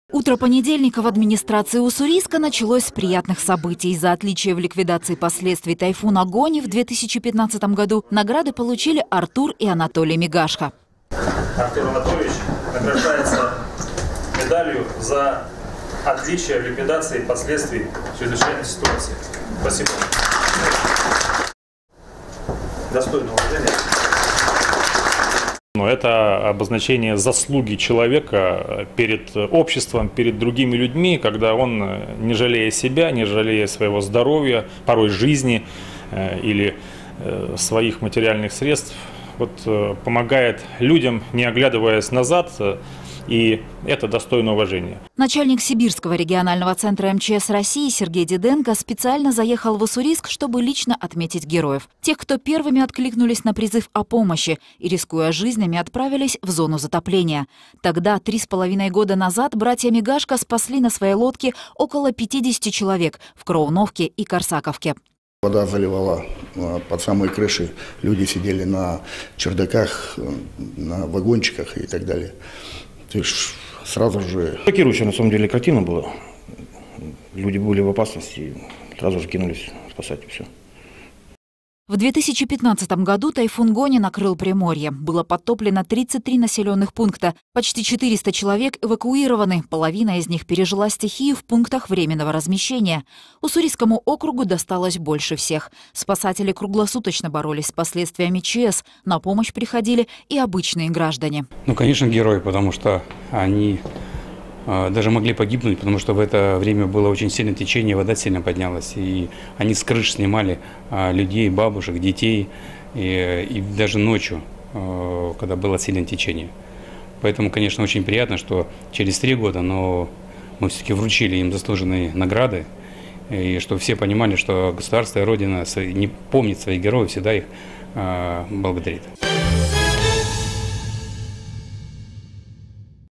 Уссурийские братья Артур и Анатолий Мигашко награждены за отличие в ликвидации последствий ЧС
Медаль через три года. Нагрудные знаки вручил глава Сибирского регионального центра МЧС России генерал-лейтенант Сергей Диденко.